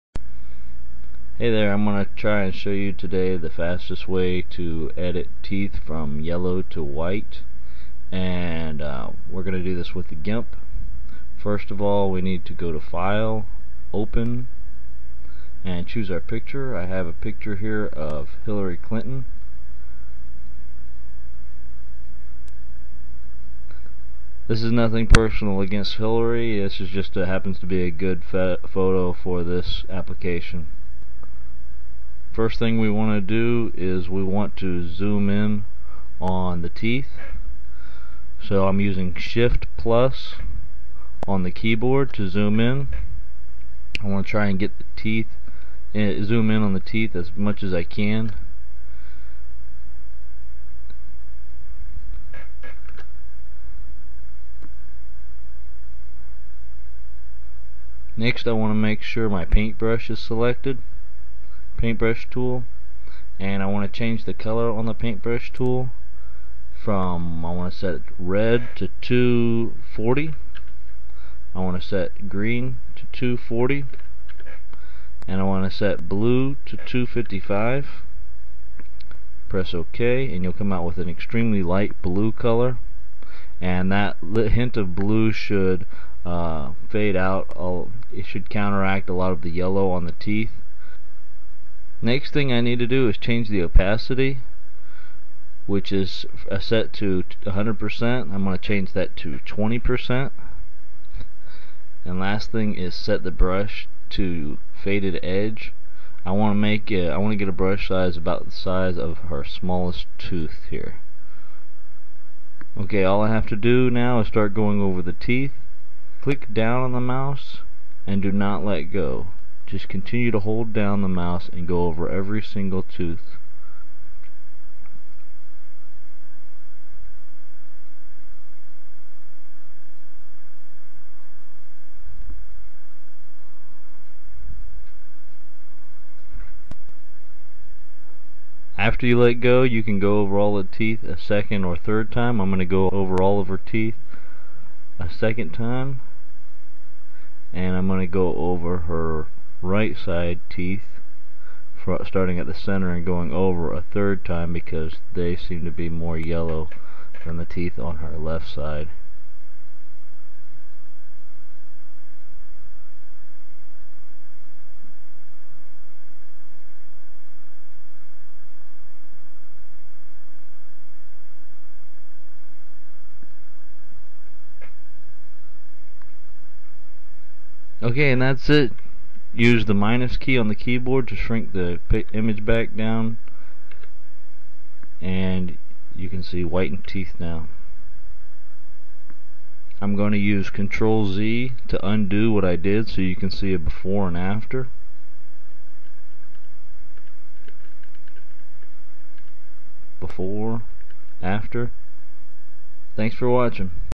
Easy Teeth Whitening Tutorial with Gimp
An easy tutorial on making teeth in your images and photos whiter. Brighten your pictures up in just a few simple steps.

Lighting can make teeth appear more yellow than they actually are.